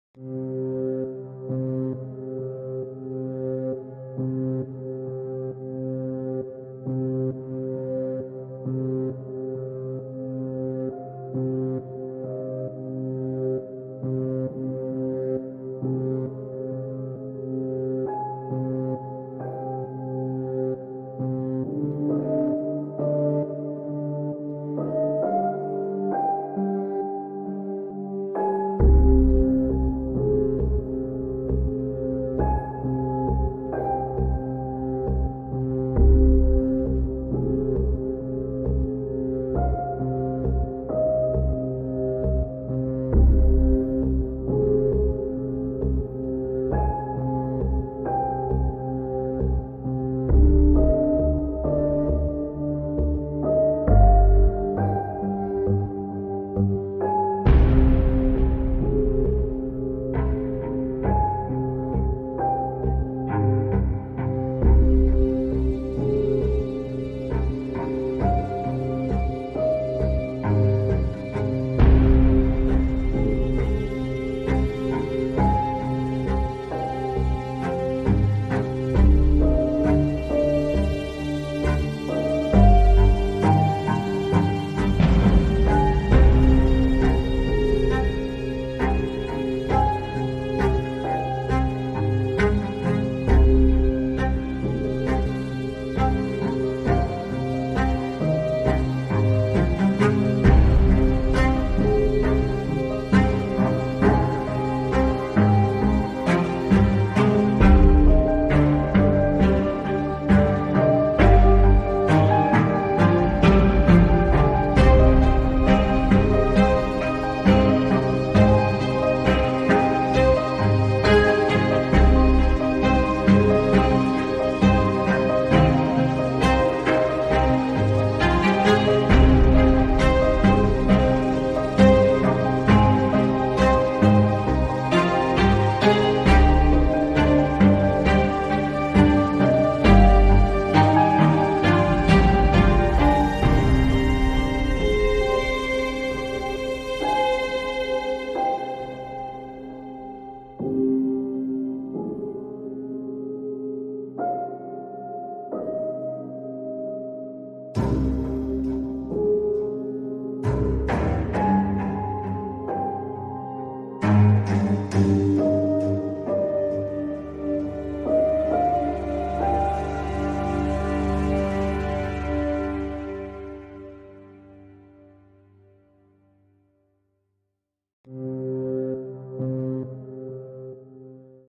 Unpredictable Trivia: Test Your Knowledge about Ancient Egypt
Explore the mysteries of ancient Egypt in this captivating trivia challenge! Test your knowledge with 10 intriguing questions about pharaohs, pyramids, and hieroglyphics. Can you unlock the secrets of the Nile? Let's dive into the sands of time and embark on a journey through Egypt's rich history.